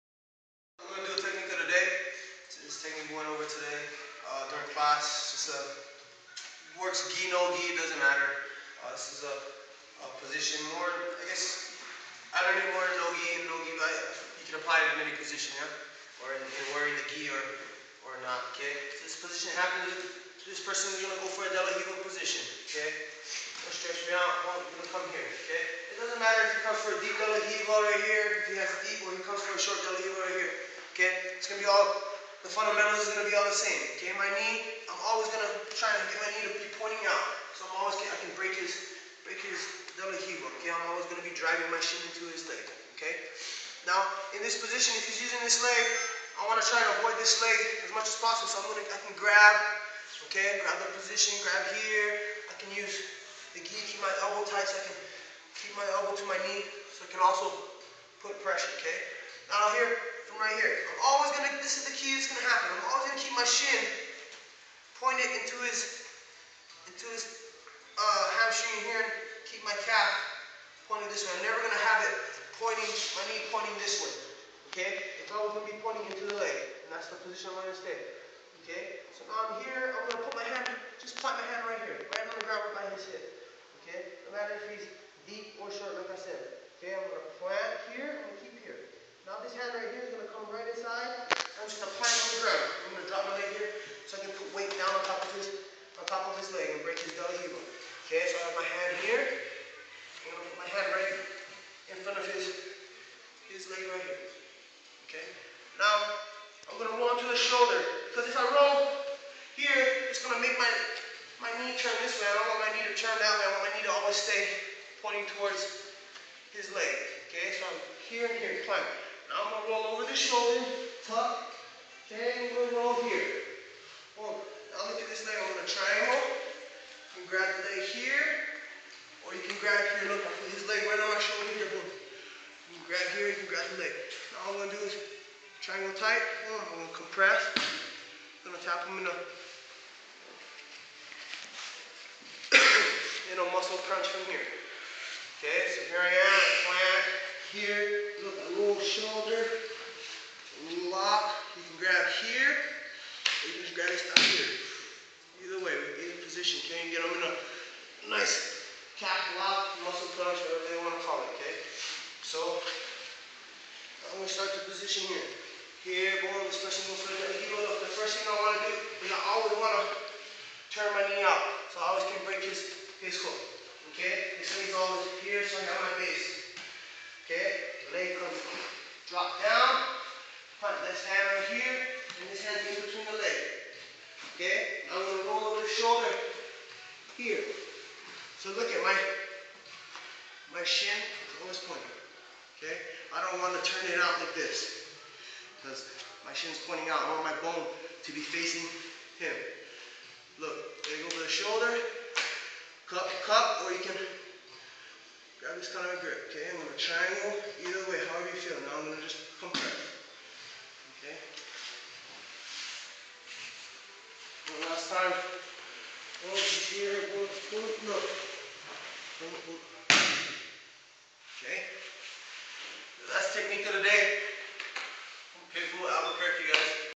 Clube De PITBULL Abq technique of the day! 2/11/12
Prof. Kale showing technique. Prana kimono and PITBULL family!